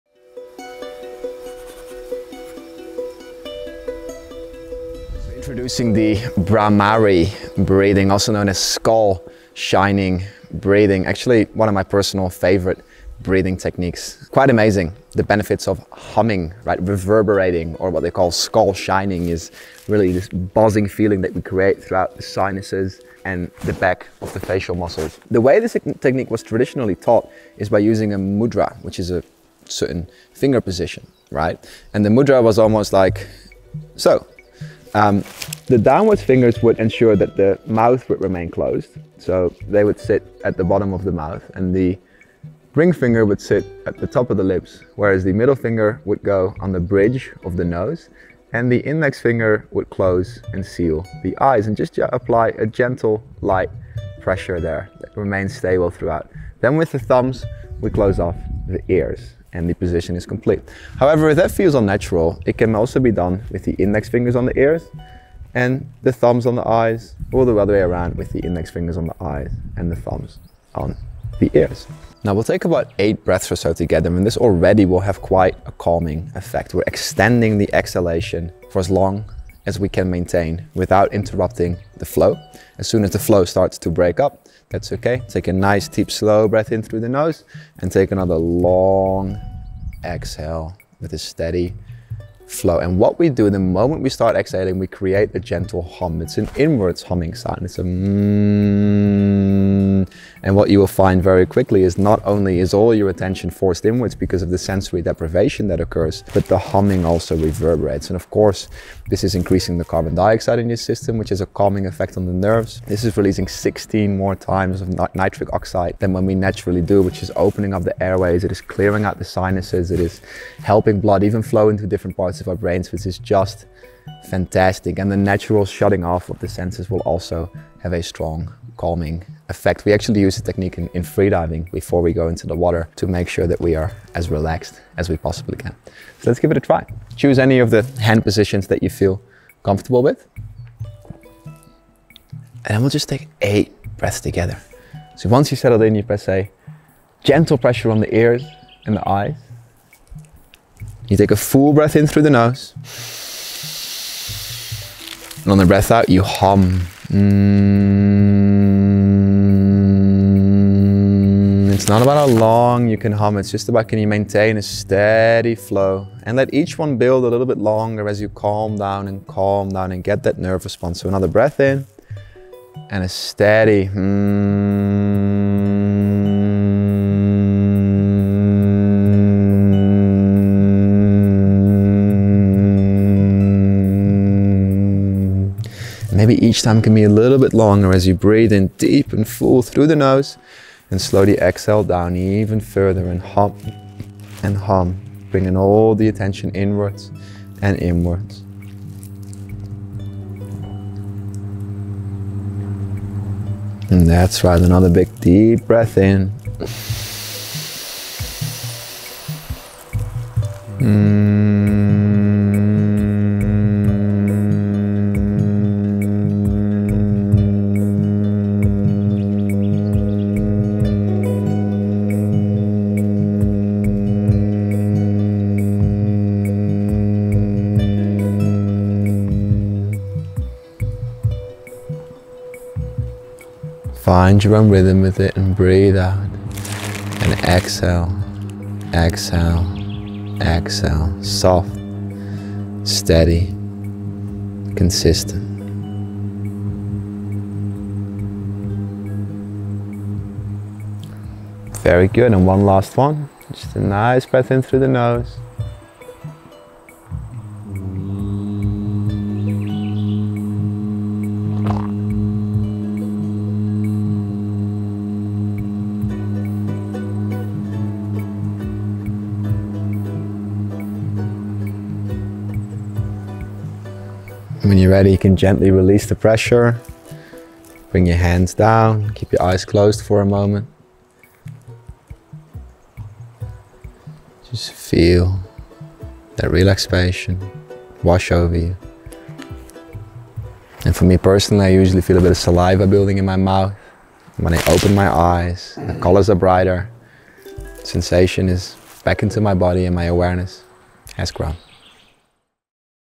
Unlocking Serenity: Mastering Bhramari Breathing Technique for Inner Calm
Are you seeking inner peace and tranquility? Look no further! In this captivating video, we will take you on a blissful exploration of Bhramari Breathing, also known as the "Bee Breath."

Join us as we delve into the incredible world of Bhramari Breathing, a breathing technique that can transform your life, and an ancient yogic practice, renowned for its ability to induce deep relaxation and inner tranquility.

By incorporating Bhramari Breathing into your daily routine, you can unlock the true potential of your mind and body, achieving mental clarity and harmony.

Learn the intricacies of this technique as we guide you through the process step by step. From the correct hand mudras to the gentle humming sound that resonates through your sinuses, we'll ensure you master each aspect for maximum effectiveness.

We will explore the profound connection between mindfulness and Bhramari Breathing, revealing how they complement each other perfectly. This powerful combination can lead to a heightened sense of self-awareness and inner calm.

Bhramari Breathing is a simple yet powerful method to release stress, reduce anxiety, and enhance overall well-being. Through the rhythmic humming sound, it enables you to focus inward, quieting the mind and finding serenity in the present moment.

You will experience the physiological effects of Bhramari breathing, from its calming influence on the nervous system, to its ability to increase nitric oxide levels and improve blood flow to the brain. Explore the sensory deprivation that fosters inward focus and deep relaxation, similar to techniques used in freediving to induce calmness before entering the water.

With our step-by-step instructions, you can easily integrate this practice into your life. Embrace a calmer mind and a more balanced existence as you cultivate mindfulness through breath.

📢 Share this knowledge with friends and loved ones seeking a more mindful and balanced existence. Remember, the power to change begins with the simple act of breathing.

🙏 Thank you for joining us on this enriching journey of self-discovery and empowerment. Let's breathe, evolve, and thrive together! 🙏